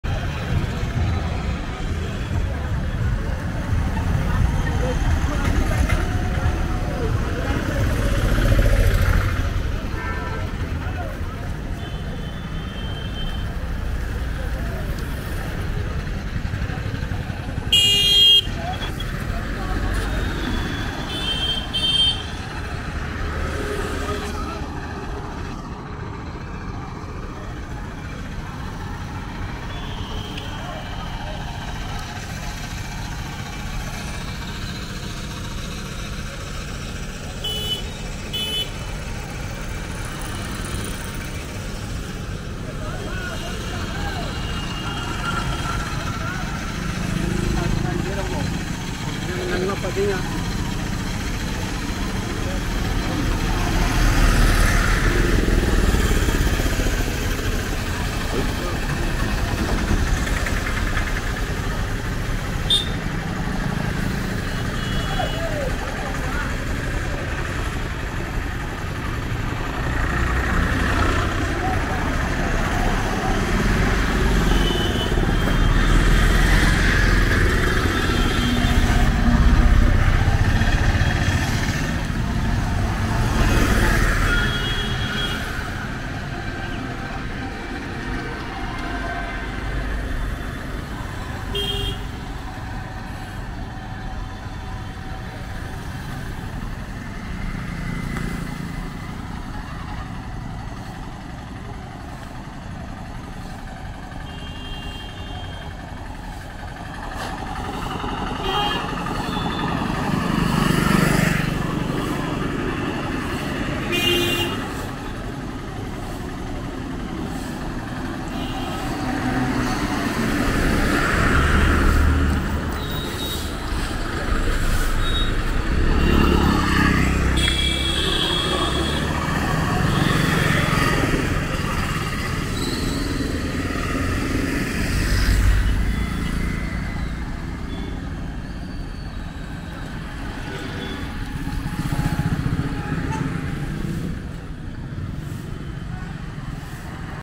MAIN ENTRY ROAD TO MAHABODHI TEMPLE | महाबोधि मंदिर का मुख्य प्रवेश मार्ग
This is the main road entrance to the Mahabodhi temple, it is close to Magadh University.